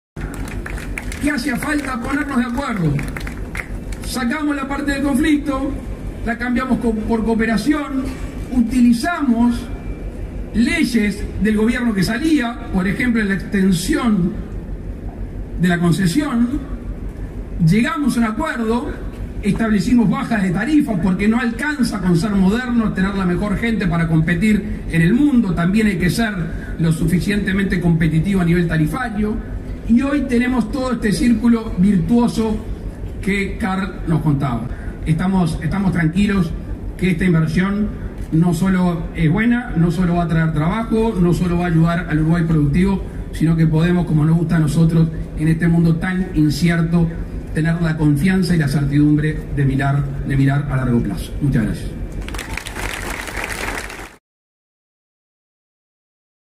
Presidente Luis Lacalle Pou en presentación de inversiones de Terminal Cuenca del Plata (TCP).
Dijo Lacalle: "Está inversión no solo es buena, no solo va a traer trabajo, no solo va ayudar al Uruguay productivo, sino que podemos, en este mundo tan incierto, tener la confianza y la certidumbre de mirar a largo plazo"